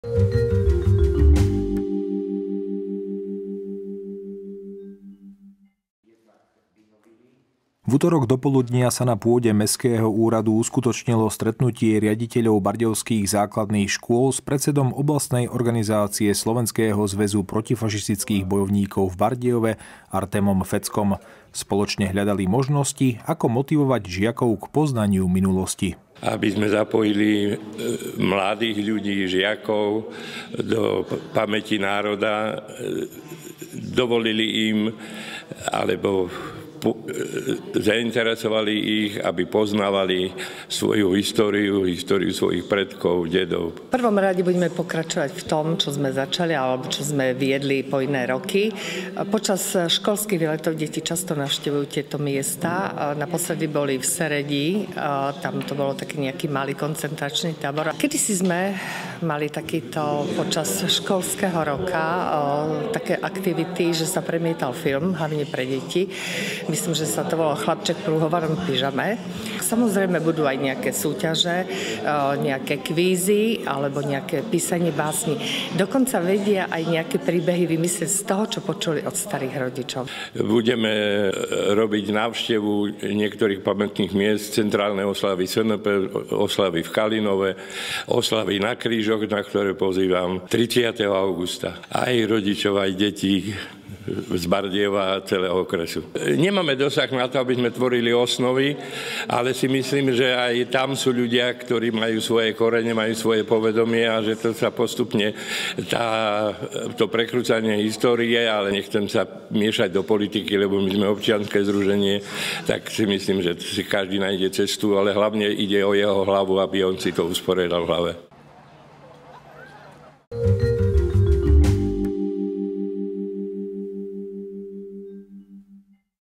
Stretnutie riaditeľov škôl k téme našich dejín
O motivácii žiakov k poznaniu histórie sa dnes hovorilo na pôde Mestského úradu počas stretnutia riaditeľov základných škôl a predsedu Oblastnej organizácie SZPB v Bardejove.